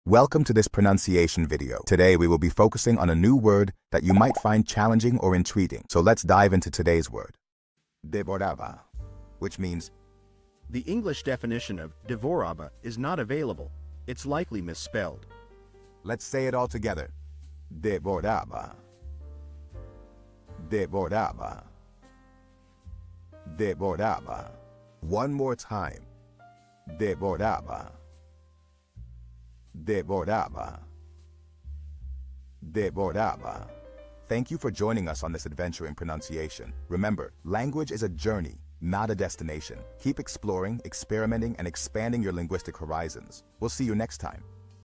How to pronounce Devoraba
Master the Pronunciation of 'Devoraba Which Means devoured' - which means : The English Definition of "devoured" is notvailable;It's Likely Misspelled. 📝 with @PronunciationSpanish 🎓🗣️ - [Your Guide to Spanish]

Learn to pronounce 'Devoraba' perfectly in Spanish with our easy and engaging tutorial by @PronunciationSpanish! 📖📽️ In this comprehensive video, we provide you with native speaker examples 🗣️👂 and phonetic breakdowns 🎵 to ensure you get the pronunciation just right.

How to say Devoured in Spanish